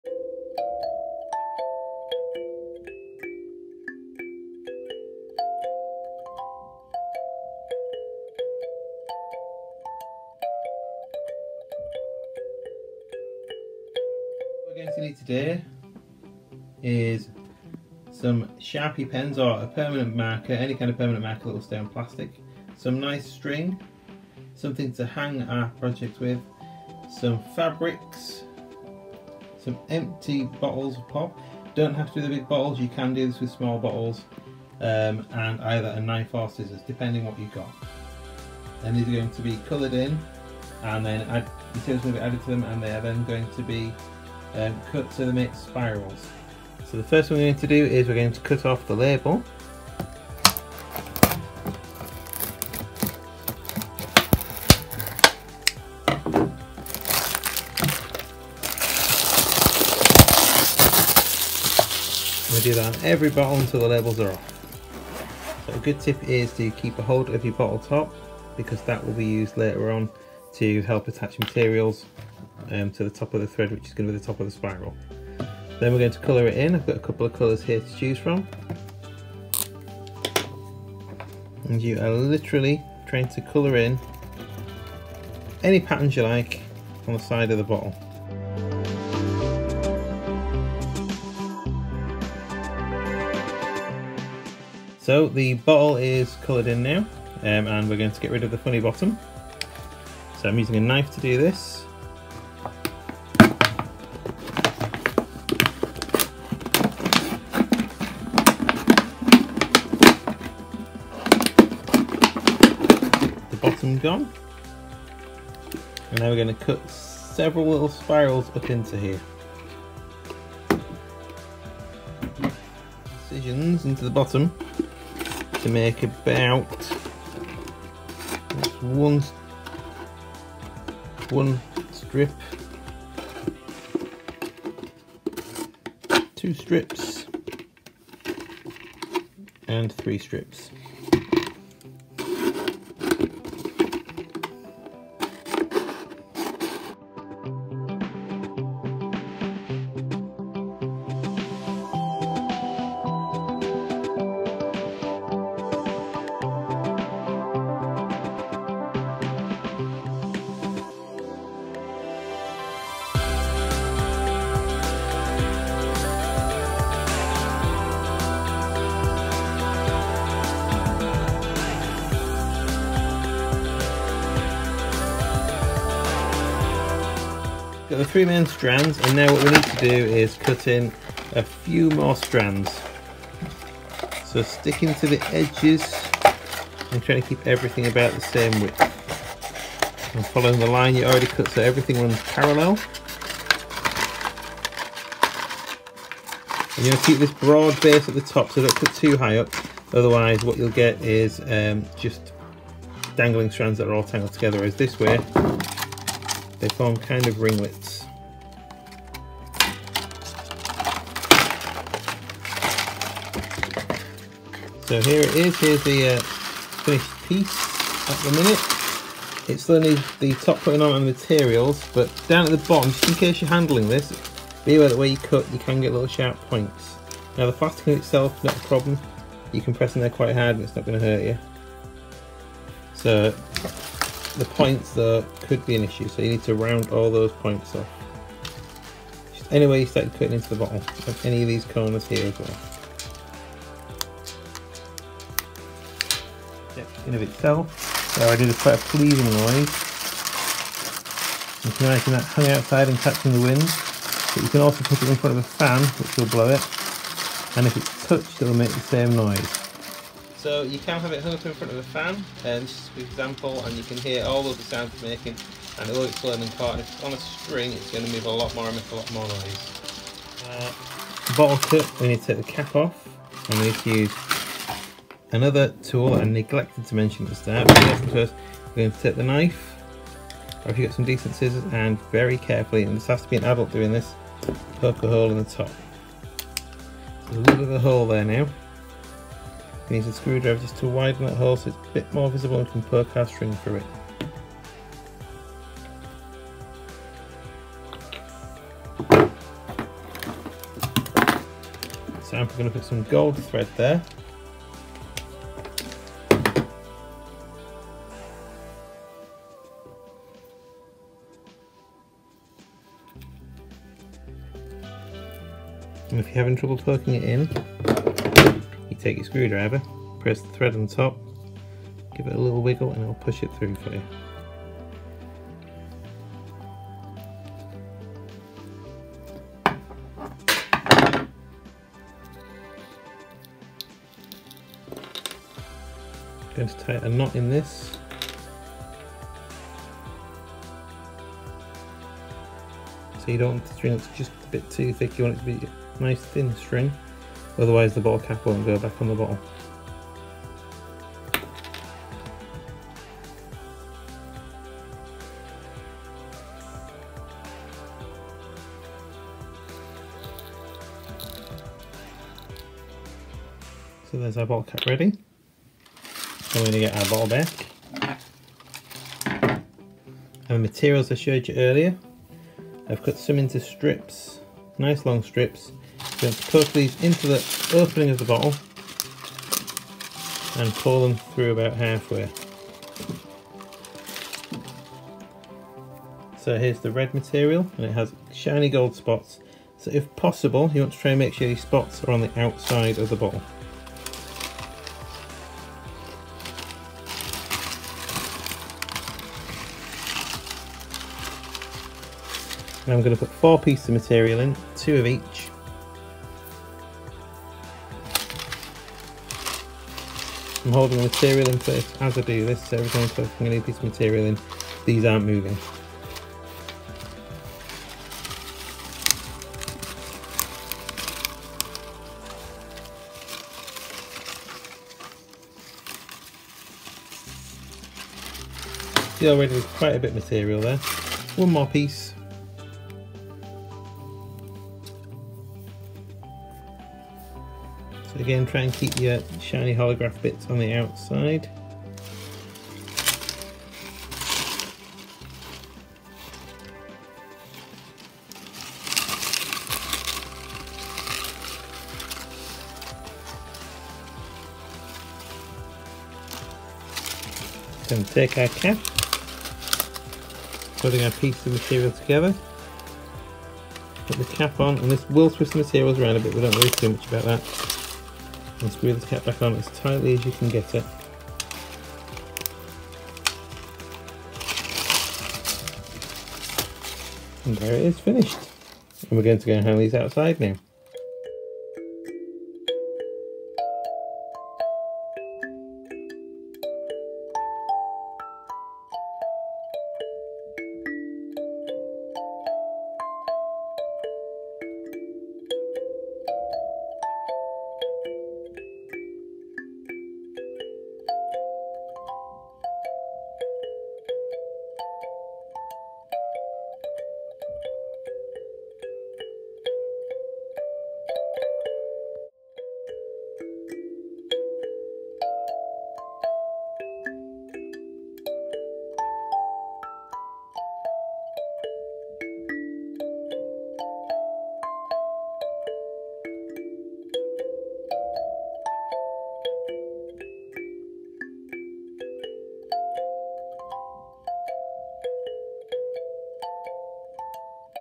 Sensory Art with Damian- Plastic Bottle Spirals
Join Damian in creating plastic bottle spirals with easily accessible resources.